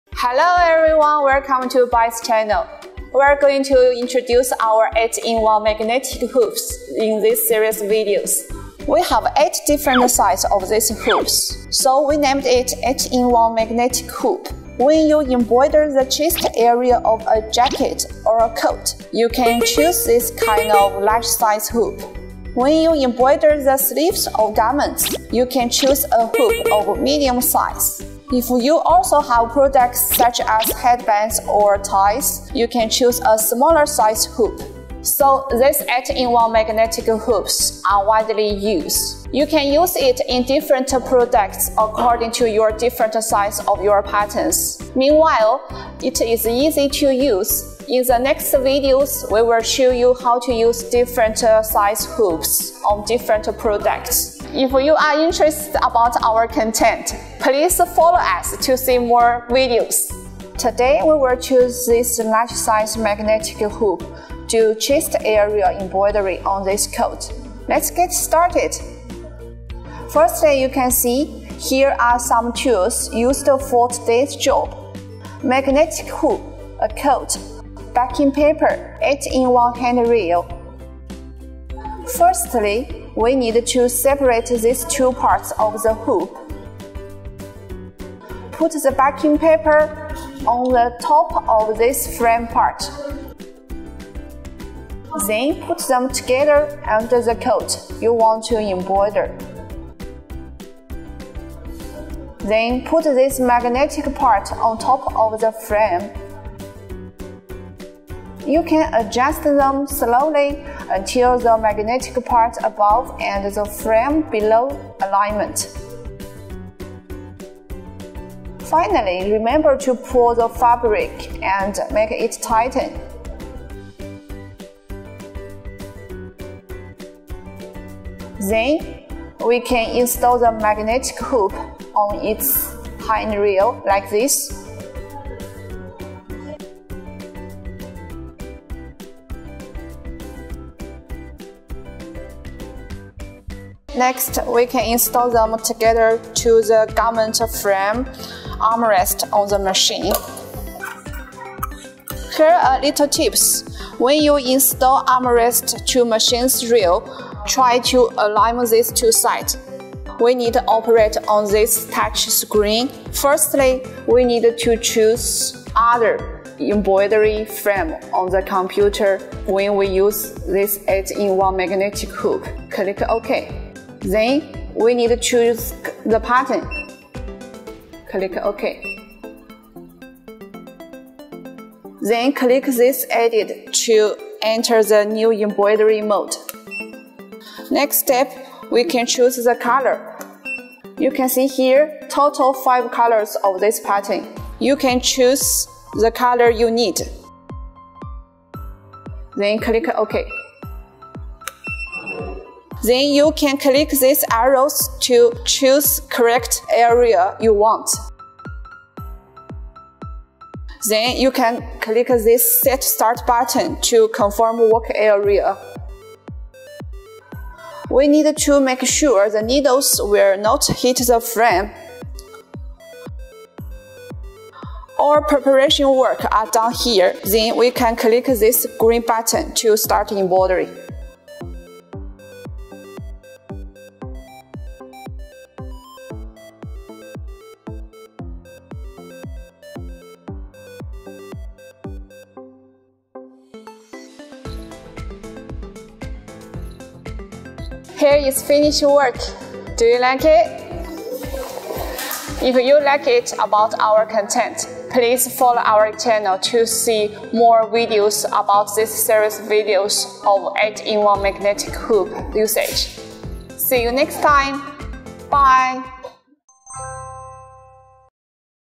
How to Embroider Chest Logo | BAI Embroidery Machine
This video will show you how to use the 8 in 1 magnetic hoop embroider chest logo.

If you have any questions, please leave a message to tell me, thank you very much! !

Here's what we'll cover in this video:
00:09 Different sizes magnetic embroidery hoops
01:02 Prepare embroidery tools and materials
01:29 Fixing backing paper and coat
02:13 Fix magnetic embroidery hoop to handrails
02:30 Installation on machine handrail
02:48 Choose the embroidery area
02:55 Choose the embroidery pattern
03:08 Choose the embroidery color
03:51 Start embroidery